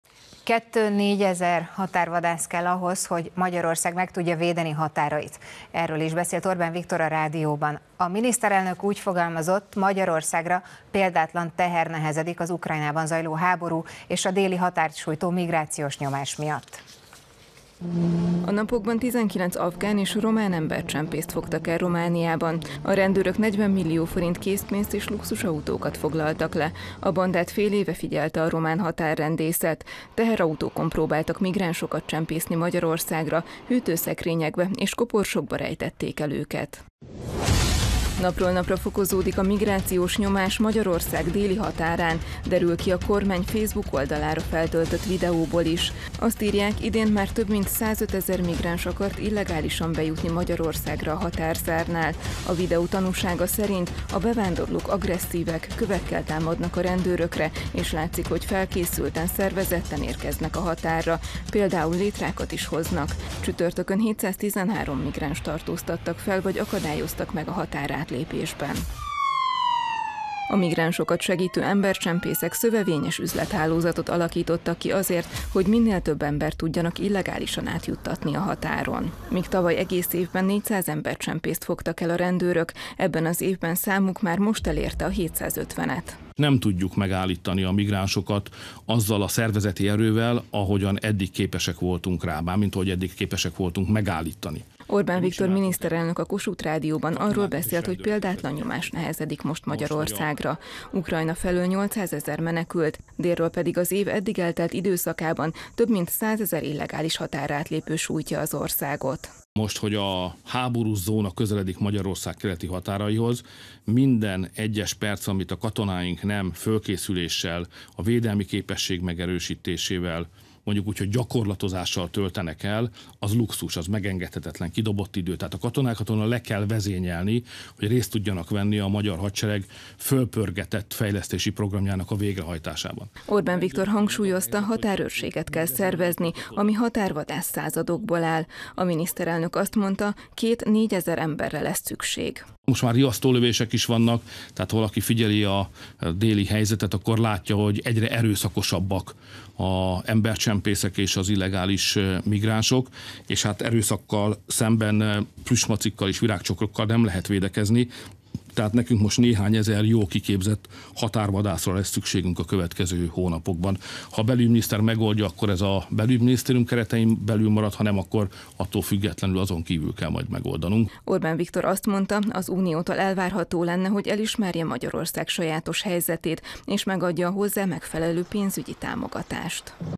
Orbán Viktor: határvadászokra van szükség
2-4 ezer határvadász kell ahhoz, hogy Magyarország meg tudja védeni határait. Erről is beszélt Orbán Viktor a rádióban. A miniszterelnök úgy fogalmazott: Magyarországra példátlan teher nehezedik az Ukrajnában zajló háború és a déli határt sújtó migrációs nyomás miatt.